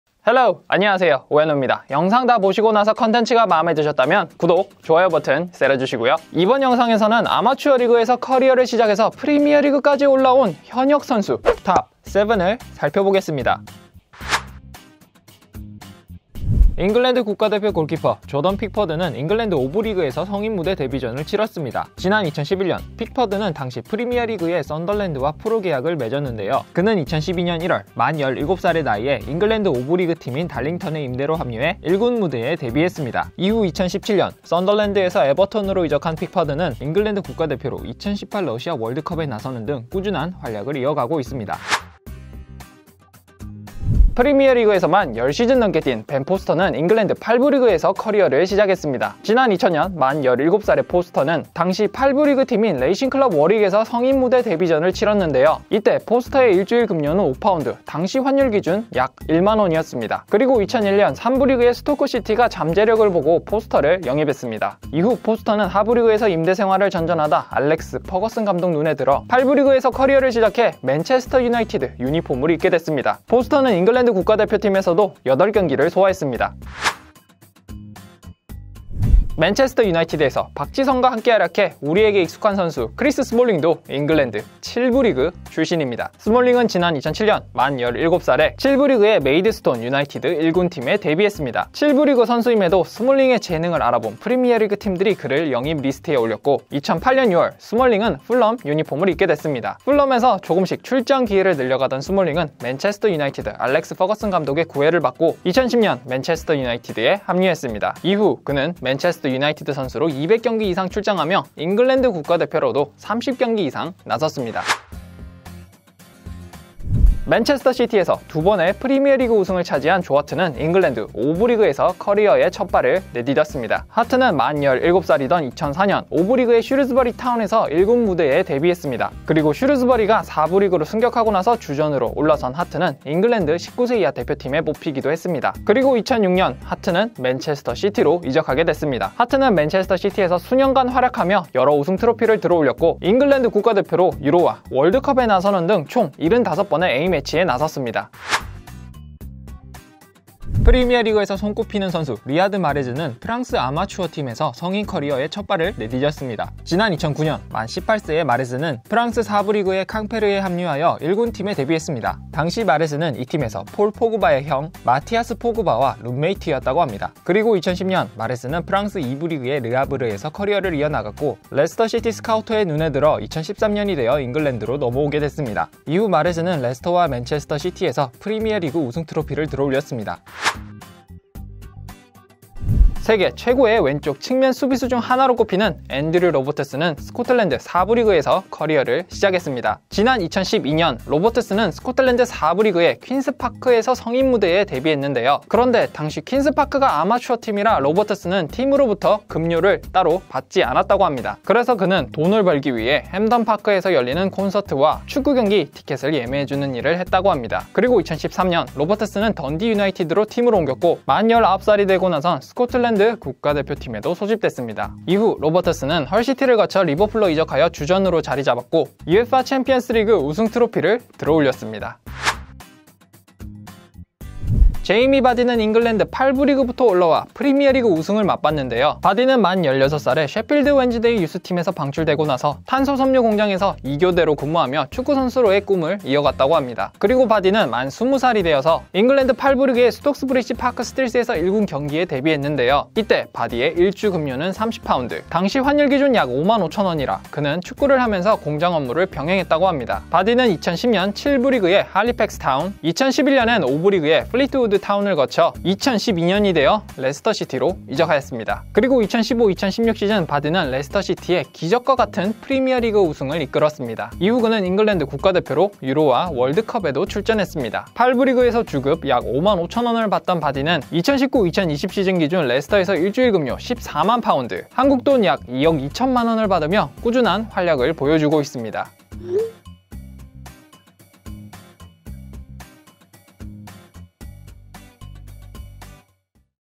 아마추어팀에서 데뷔한 프리미어리그 선수 TOP 7
안녕하세요. 우야노입니다😃
이번 영상에서는 아마추어리그에서 성인 무대에 데뷔하여 잉글랜드 프리미어리그까지 올라온 현역 선수 TOP5 를 살펴보겠습니다!